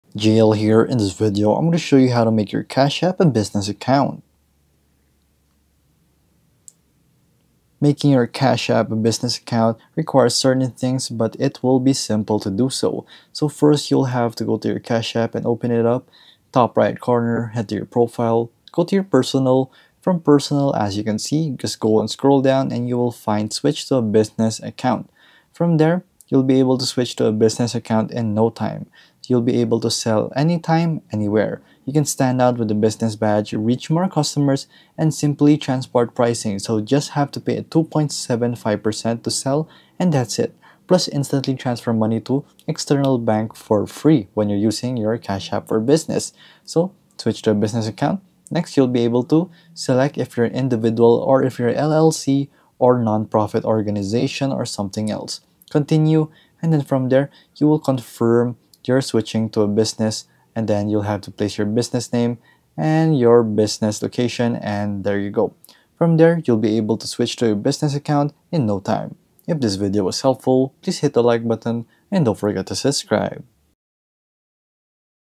How To Make Your Cash App A Business Account | Quick & Easy! (2025)
👉 In this tutorial, we will show you how to easily transform your Cash App into a business account. Learn how to make the switch, set up your business profile, and start accepting payments with your new Cash App business account.